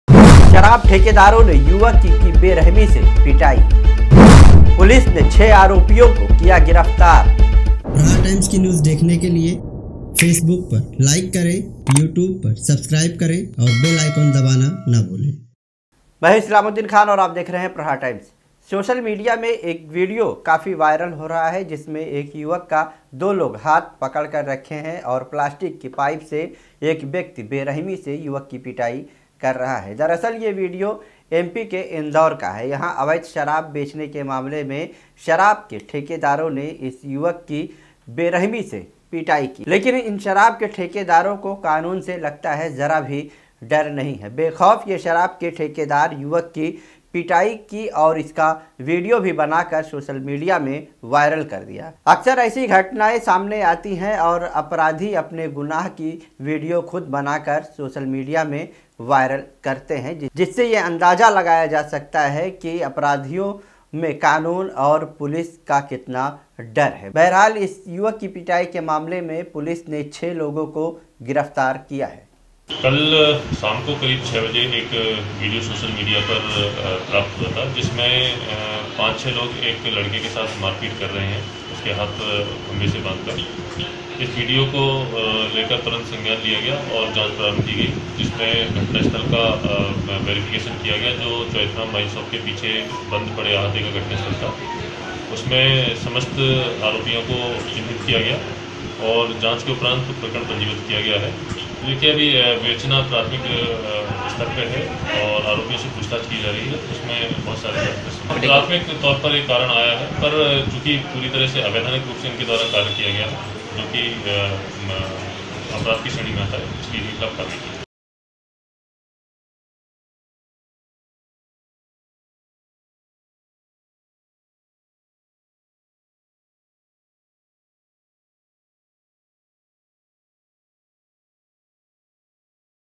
बेरहमी से शराब ठेकेदारों ने युवक को पीटा | Islamuddin Khan |Aapka Prahar Times
बेरहमी से शराब ठेकेदारों ने युवक को पीटा | Islamuddin khan |